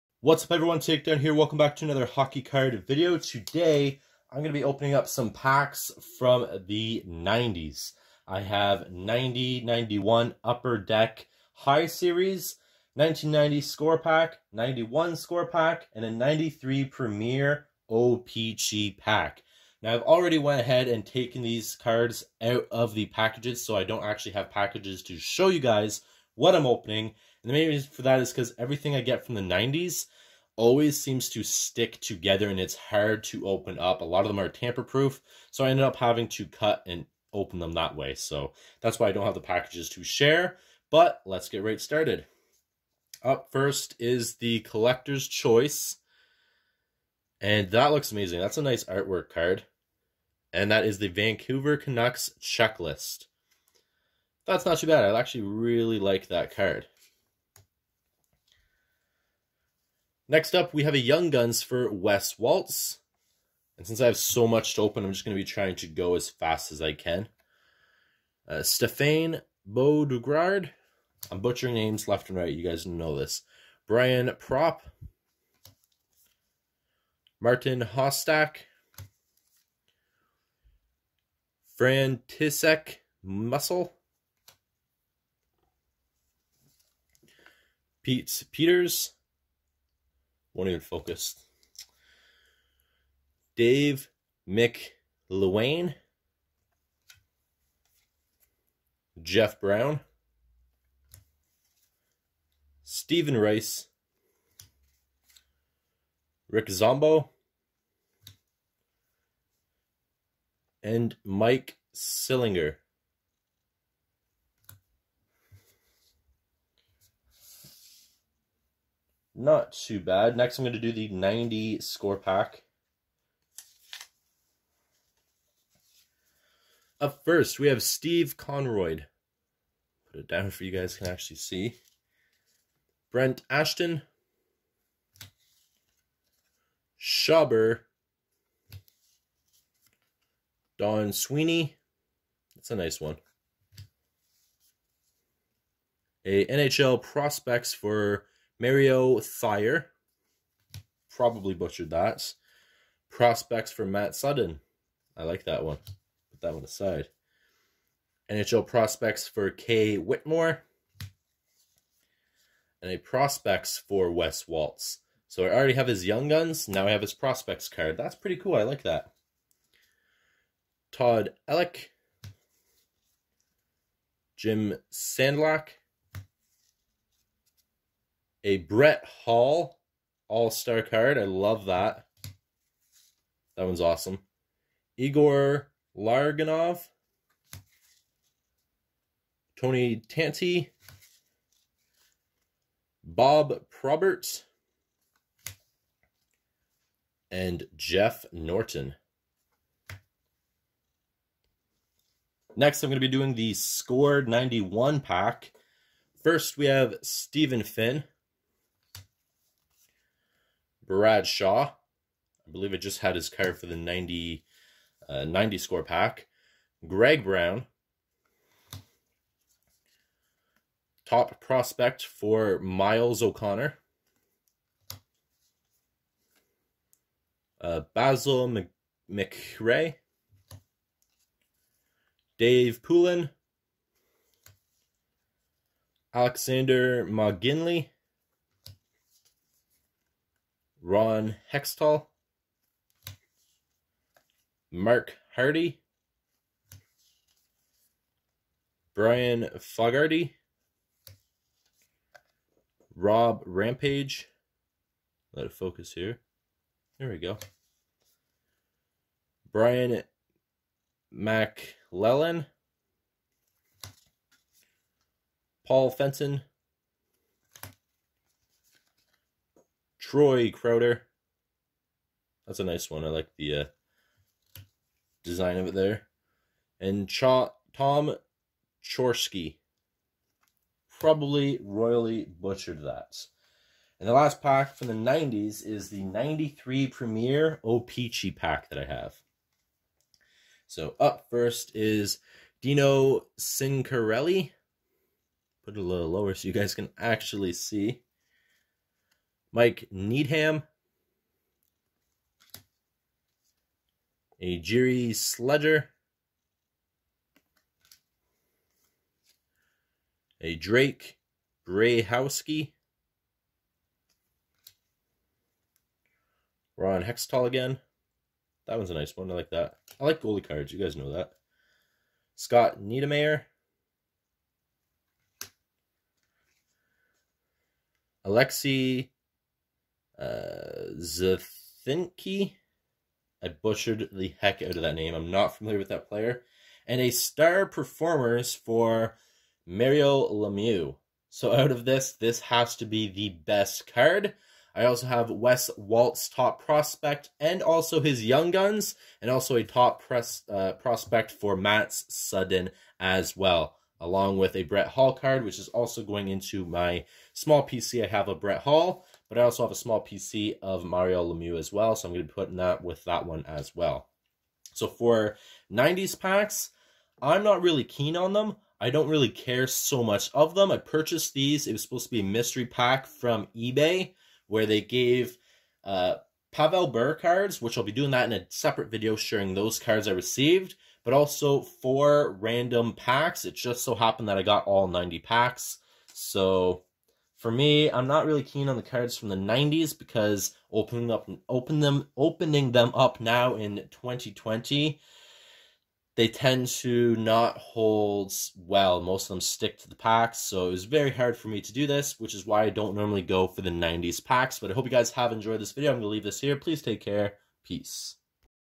HOCKEY CARD PACKS FROM THE 90's
Today we open some 90's packs of Hockey Cards! Wish me luck!